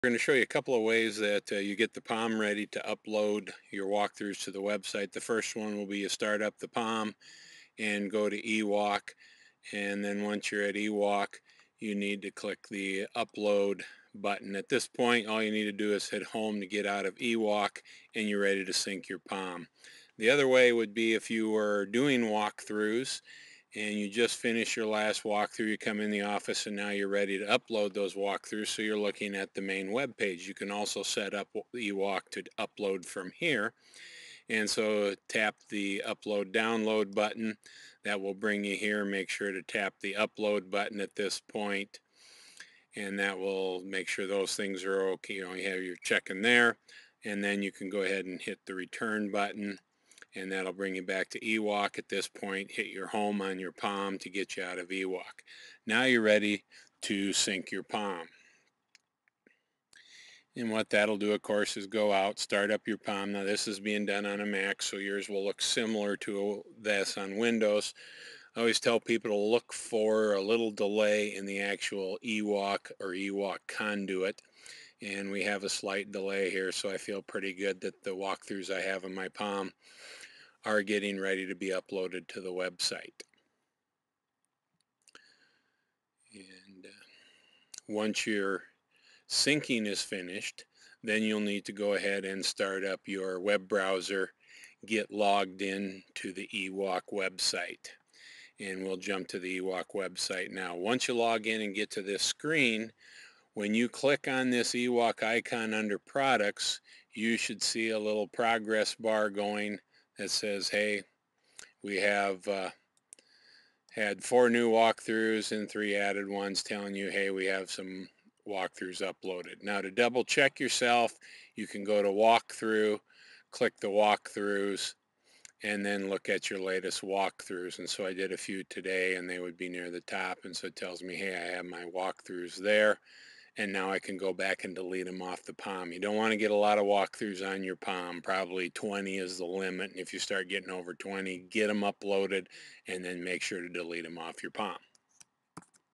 Uploading Completed Walkthroughs on your Palm to eWalk website
This video will show step-by-step instructions for uploading your completed walkthroughts to the eWalk Website from your Palm.